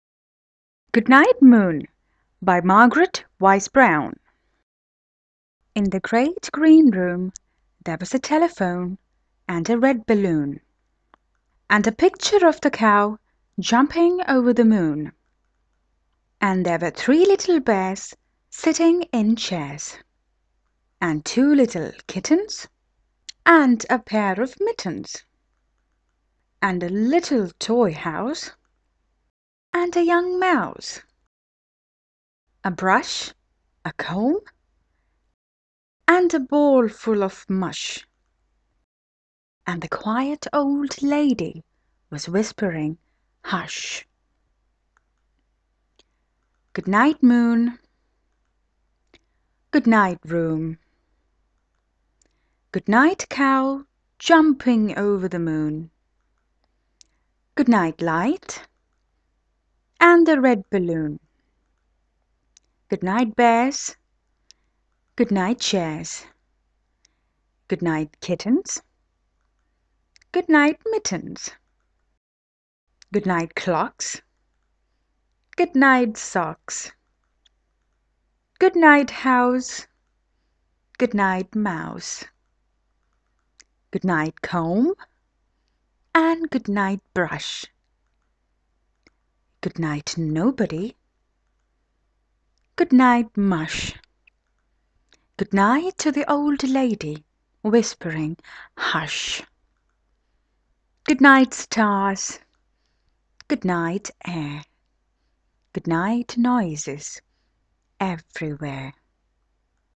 Goodnight Moon by Margaret Wise Brown - narration with pictures
One of my favourites for babies. Like other works, this one too was started as a .PPT, then I thought of adding my narration and voila! Here it is. It's a bit slow and amateurish like others, but, ah whatever! I shall improve. ;-). All pictures are done in Photoshop.